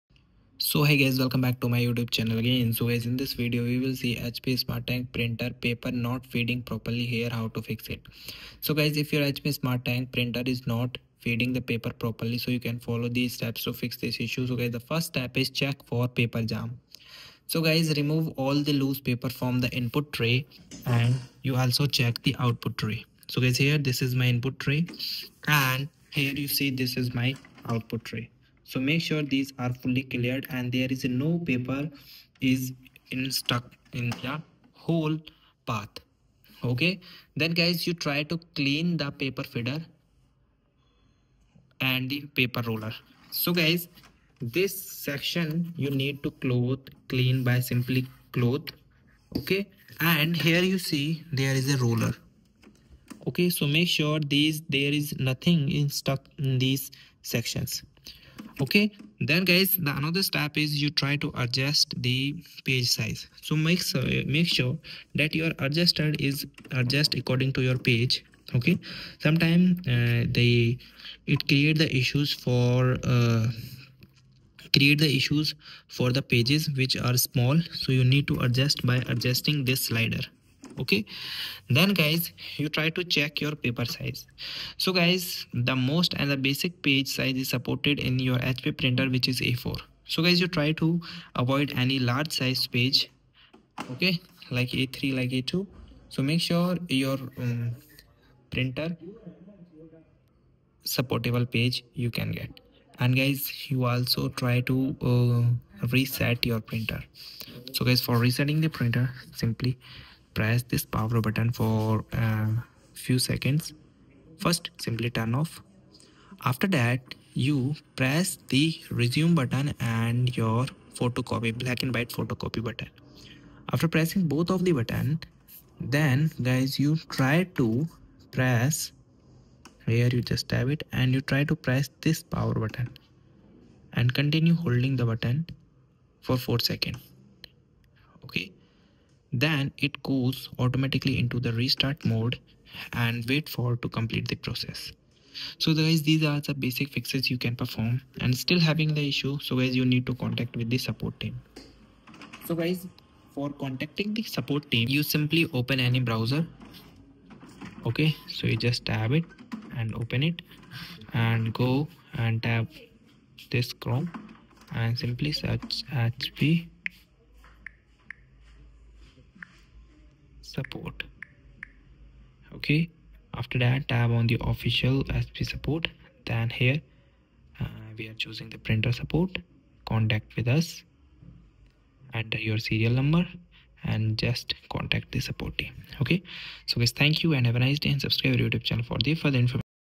HP Smart Tank Printer Paper Not Feeding Properly? Here's How To Fix
In this informative video, we address a common issue faced by HP Smart Tank Printer users: paper not feeding properly. We will guide you through a series of troubleshooting steps to identify and resolve the problem effectively. From checking the paper alignment to cleaning the feed rollers, our expert tips will help you get your printer back in working order. Whether you're printing important documents or cherished photos, ensuring your printer functions smoothly is essential. Watch now to learn how to fix your HP Smart Tank Printer and enhance your printing experience.

0:16  Fix 1: CHECK PAPER  JAM
0:40  Fix 2: CLEAN THE PAPER FIDDER
01:00 Fix 3: Adjust The Paper Sizer
01:47 Fix 4: Reset The Printer
2:30  Fix 5:Contact Support Team